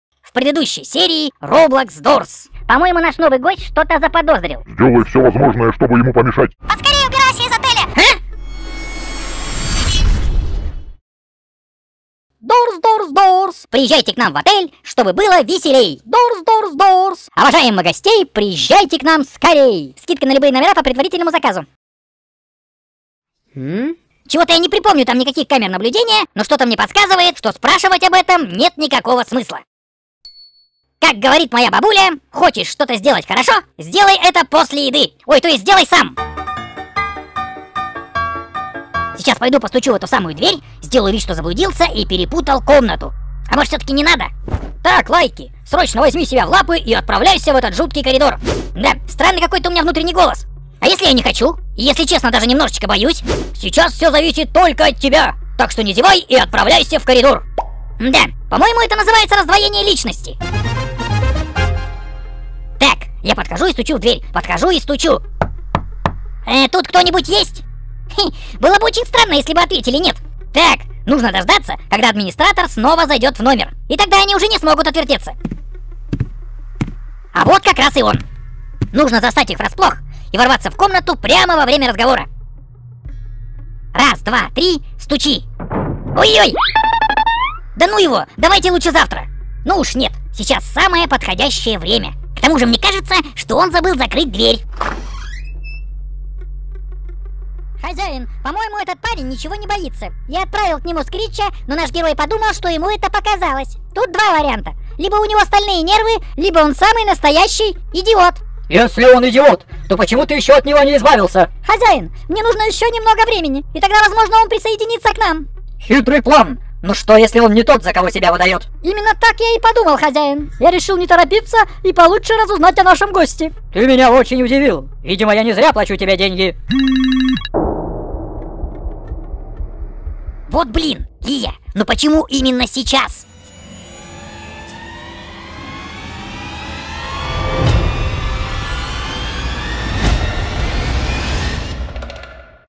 👁️ Roblox Doors 3 | Большая Анимация Роблокс Дорс
История роблокс дорс часть 3. Анимация роблокс Дорс.
Scary story roblox doors part 3. Animation roblox doors.